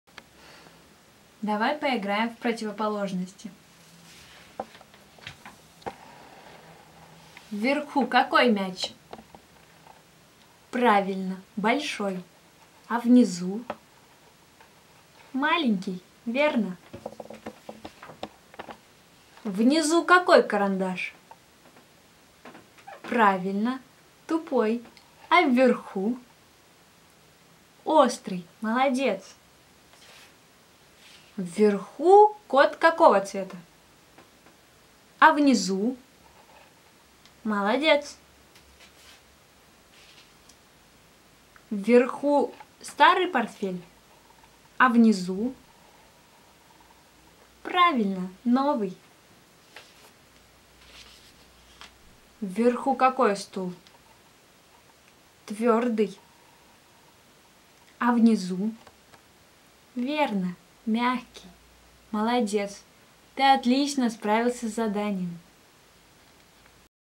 Дидактическая игра "Противоположности"
Цель:
-развивать речь, внимание, образную и смысловую память,
-закладывать основы логического мышления.